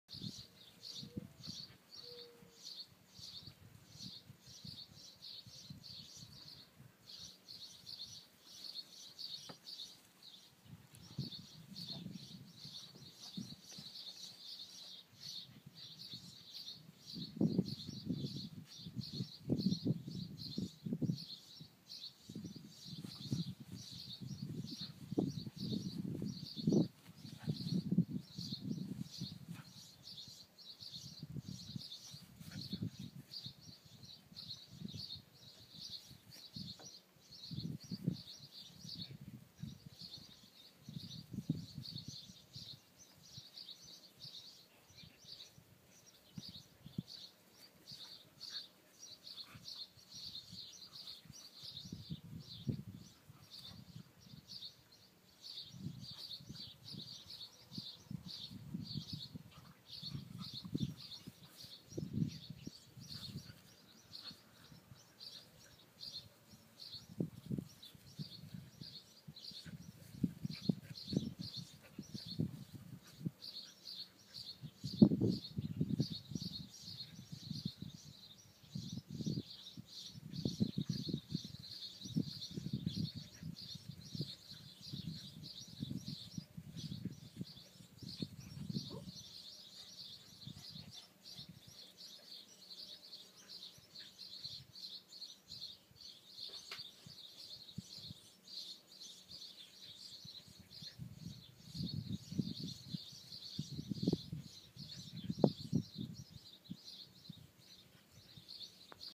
A pig and her dog
Best Friends Mini Pig and Boarded Collie play together.